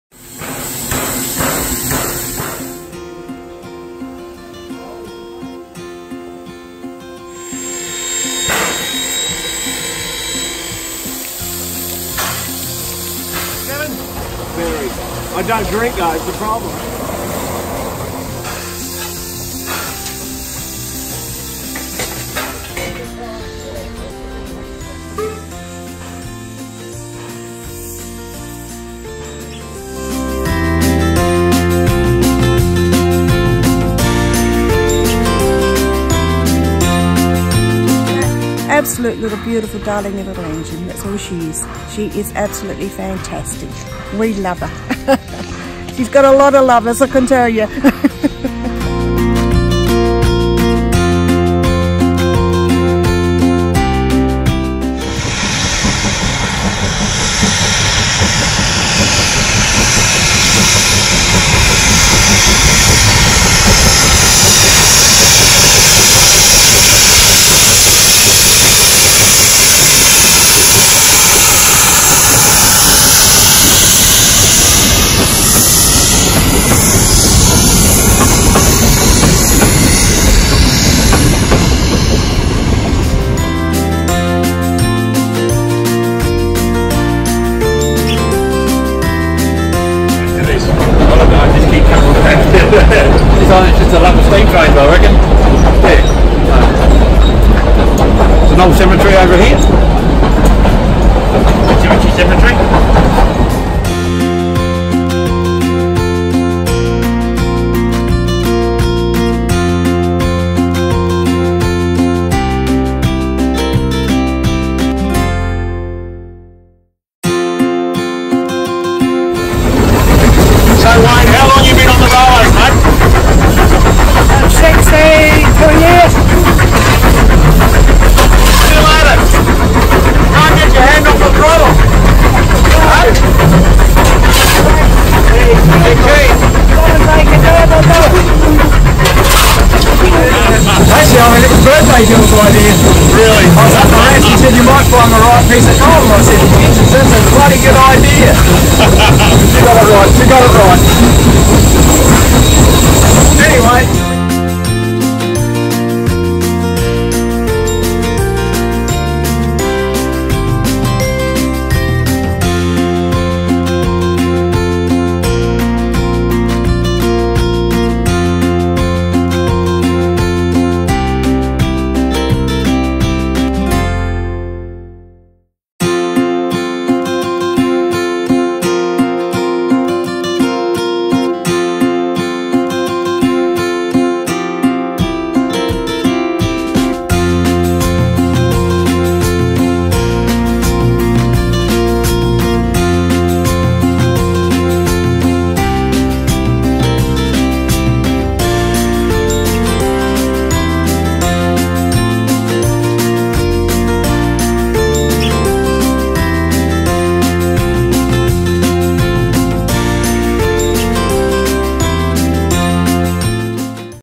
Railway Tours in Outback Australia on the Pichi Richi Steam Train
Plan your next rail tour of Australia with Scott McGregor's Railway Adventures. Fully escorted rail packages aboard the Pichi Richi.